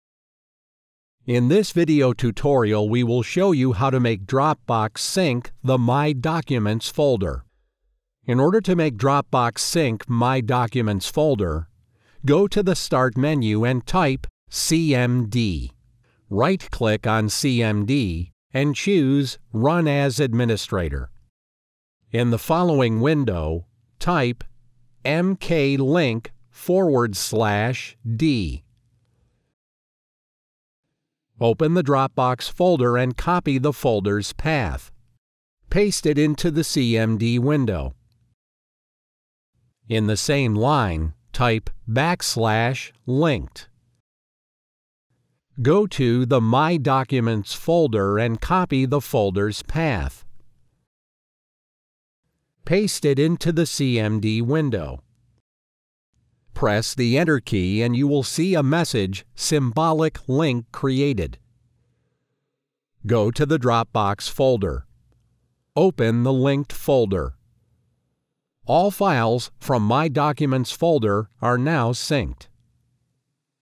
How to Make Dropbox Sync My Documents Folder
Learn how to sync "My documents" folder with the help of "Dropbox".

In this video tutorial we will show you how to make dropbox sync "My documents" folder.
In order to make dropbox sync "My documents" folder, go to the "Start" menu and type "cmd". Right-click on the "cmd" and choose "Run as administrator".

In the following window, type "mklink /d".

Open the "Dropbox" folder and copy the folder's path.

Paste it into "cmd" window. In the same line, type "\linked". Go to "My Documents" folder and copy the folder's path.

Paste it into "cmd" window. Press "Enter" key and you will see a message - "symbolic link created".

Go to the "Dropbox" folder. Open the "linked" folder. All files from "My documents" folder are synced.